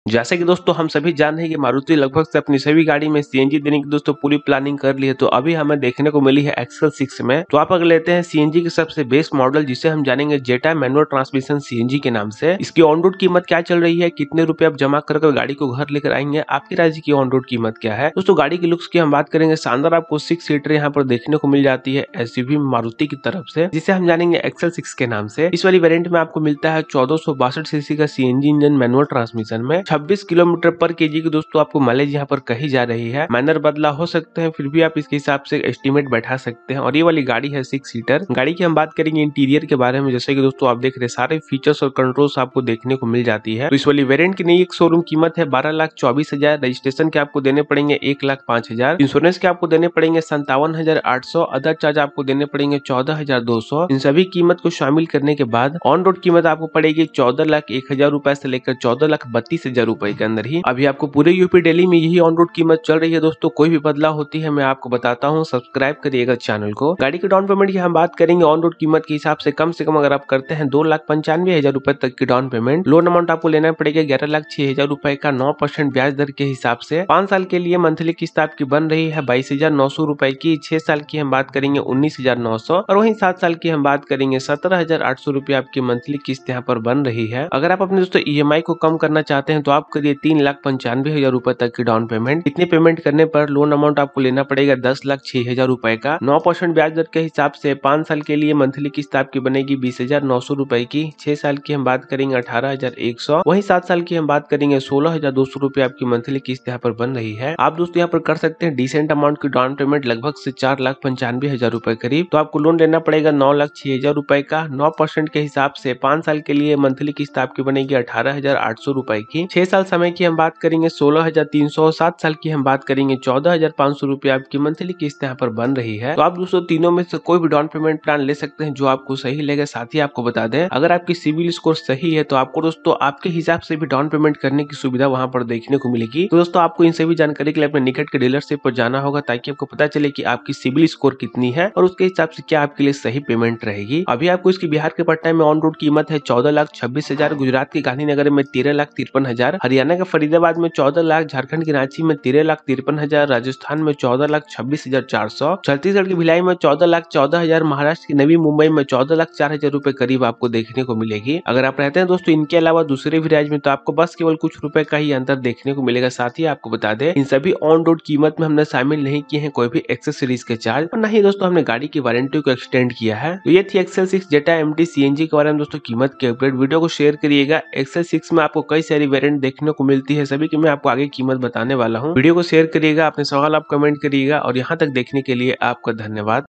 2023 Maruti Suzuki XL6 Price | Maruti XL6 Zeta MT CNG Onroad Price, 3 Downpayment, Emi, Loan Price
2023 maruti suzuki xl6 zeta mt cng price. Also explain Maruti xl6 zeta mt cng Onroad Price &  best 3 downpayment plan with 5 to 7 years monthly emi.

Before buy 6 seater SUV jane maruti xl6 zeta cng price list and bank loan finance details.